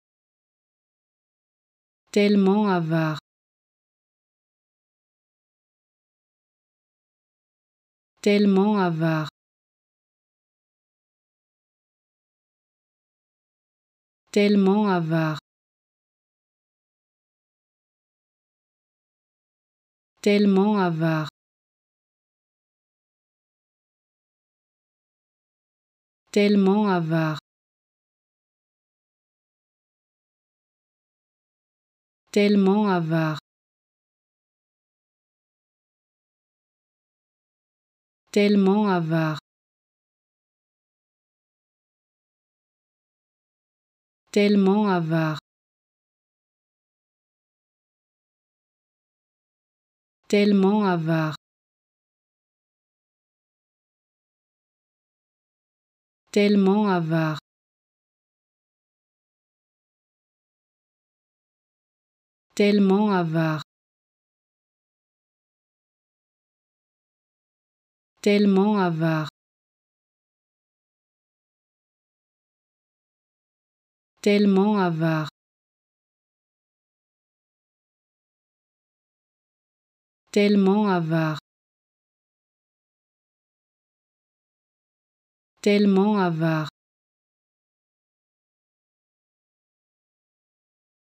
How to Pronounce ''Tellement avare'' (So stingy) Correctly in French
Learn how to say and properly pronounce ''Tellement avare'' in French with this free pronunciation tutorial.